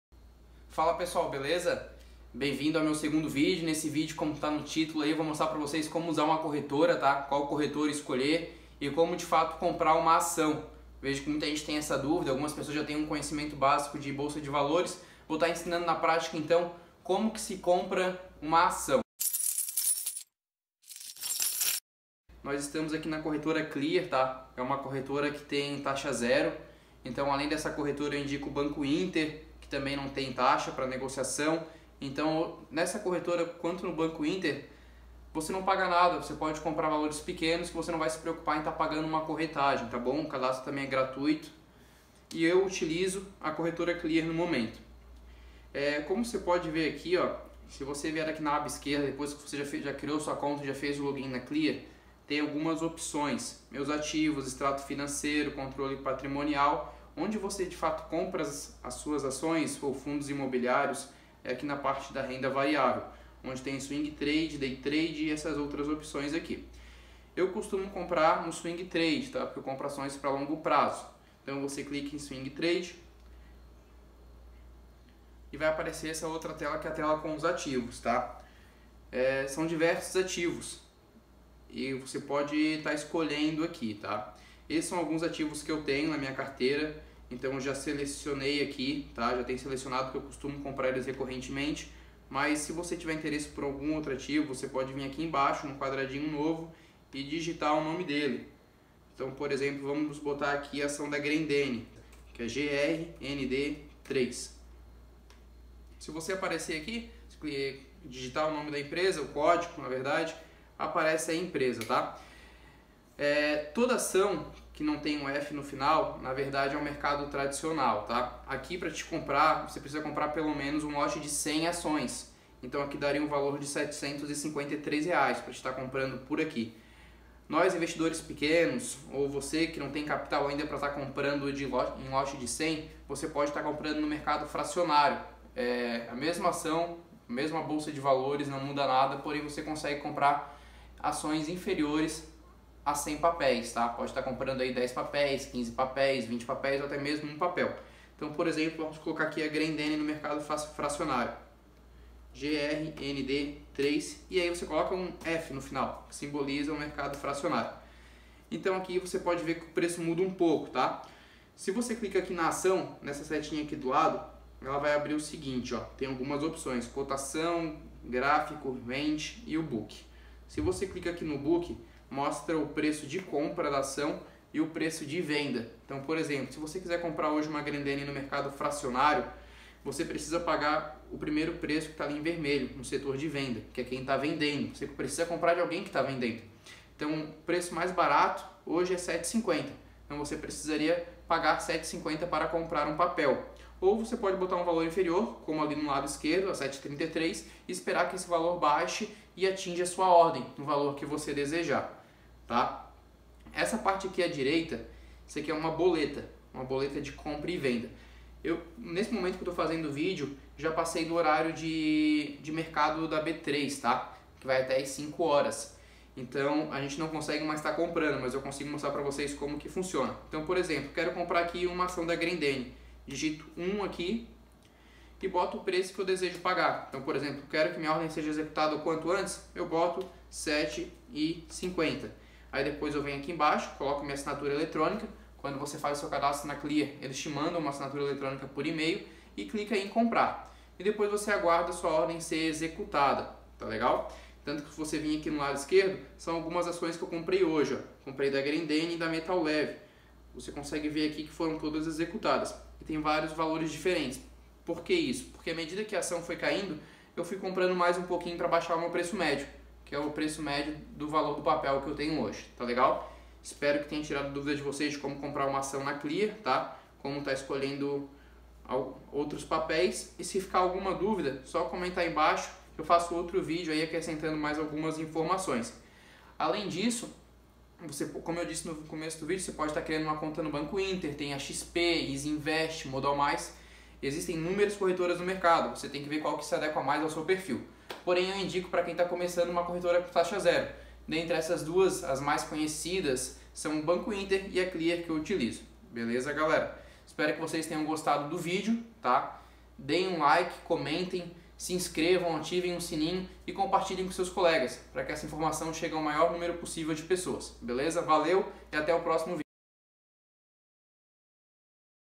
Como comprar minha primeira ação?
Esse vídeo é para você que nunca comprou uma ação na bolsa de valores.
Trago um pouco da minha experiência em comprar ações no Home Broker da corretora Clear, além de mostrar na prática como verificar o preço de uma ação no booking de ofertas.
Meu objetivo é te ajudar a perder a insegurança ao comprar uma ação do ibovespa, mostrando como é simples investir em uma corretora.
Para ilustrar melhor, comprei uma ação da empresa GRENDENE no mercado fracionário, utilizando apenas R$7,00.
Ou seja, está nítido que não precisa ser rico para começar a investir, basta apenas querer e cultivar esse importantíssimo hábito.

Grande abraço!